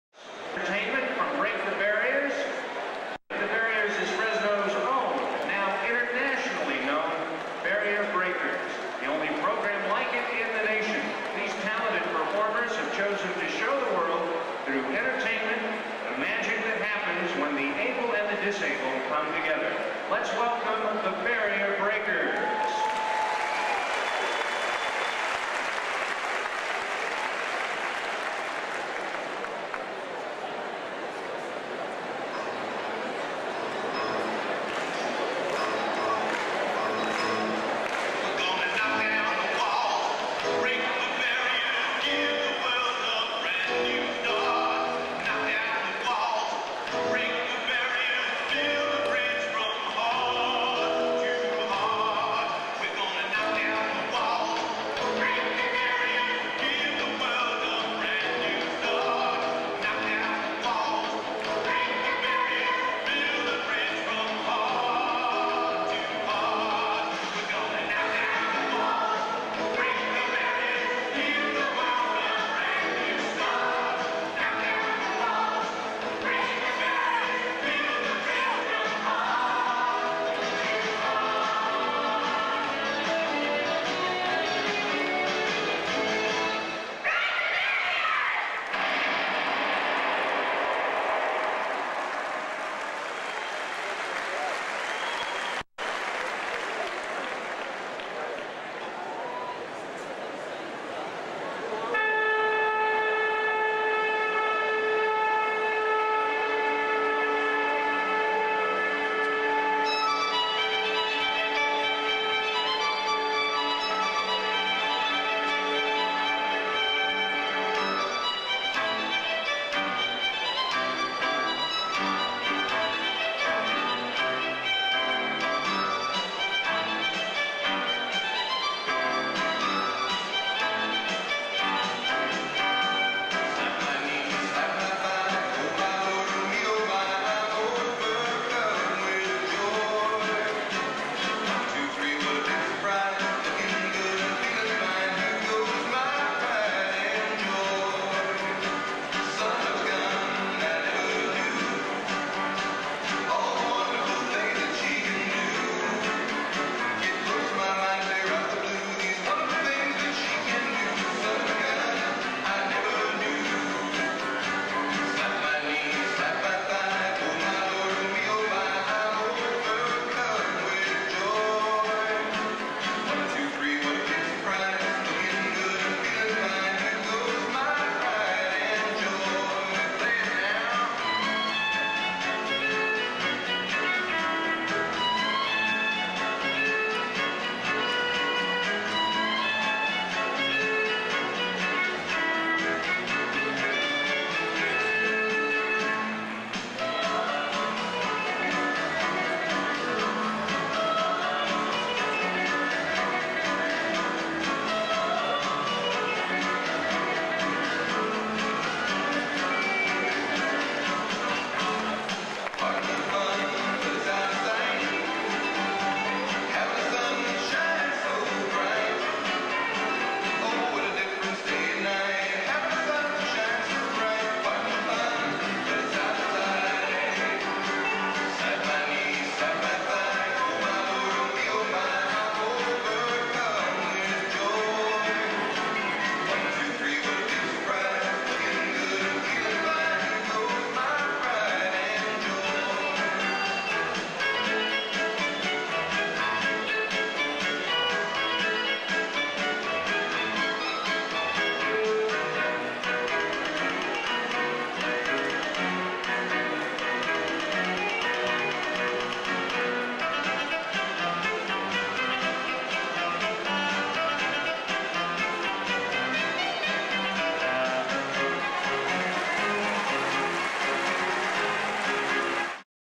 Barrier Breakers Perform at Fresno State Half Time - 1997
Each year, the Barrier Breakers perform at a Fresno State Basketball game half time.  Here is the performance from January 1997.

To learn more about Break the Barriers, visit breakthebarriers.org